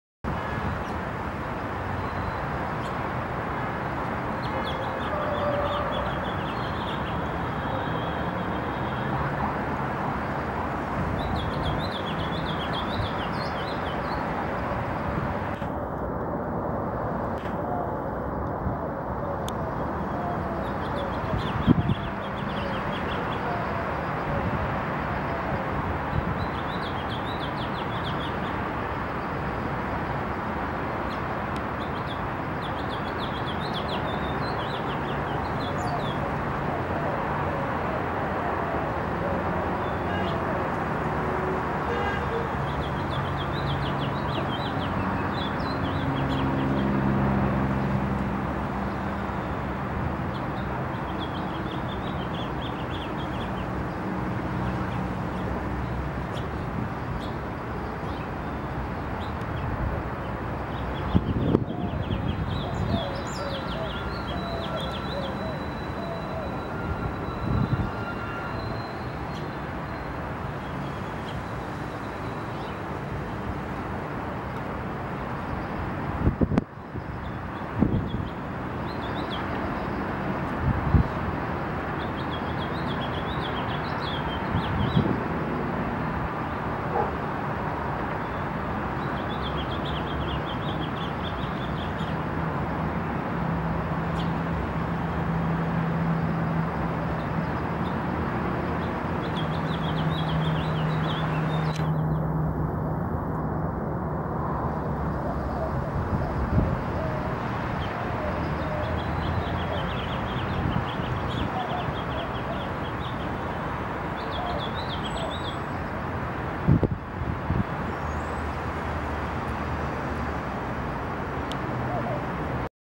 El canto del pinzón mexicano.
Por la mañana un pinzón canta sobre el nogal sin hojas, acompañado del sonido del tráfico de la ciudad y palomas.